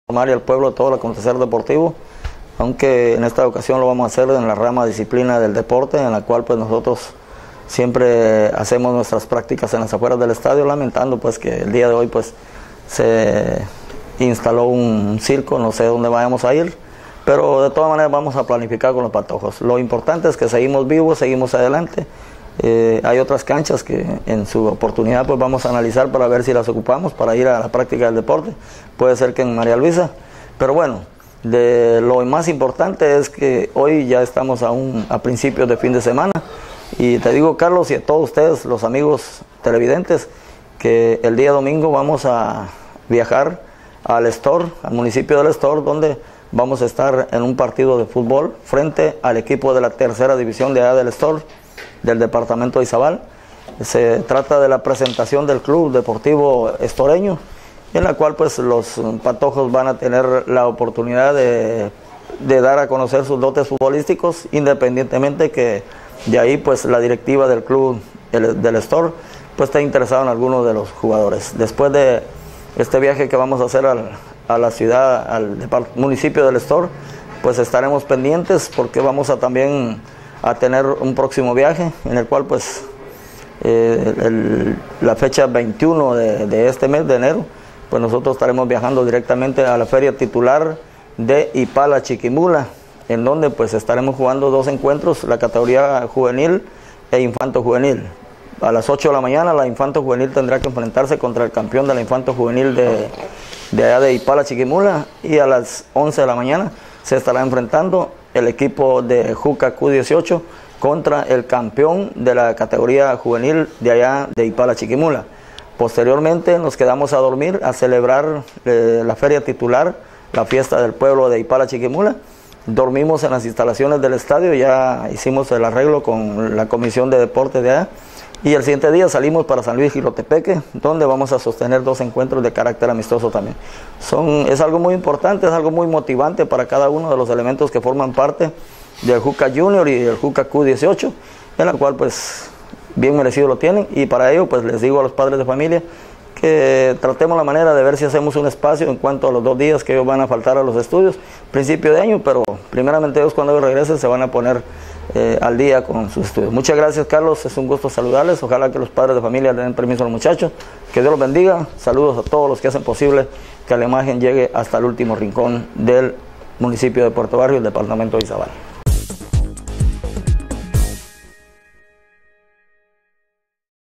PROMOTOR DEPORTIVO INFORMA A LOS PADRES DE FAMILIA QUE ESTARAN VIAJANDO A ZACAPA
TRANSMITIDO POR CABLE BARRIOS ATRAVES DE CANAL 9 PUERTO TV COMUNICATE CON NOSOTROS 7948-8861/56320701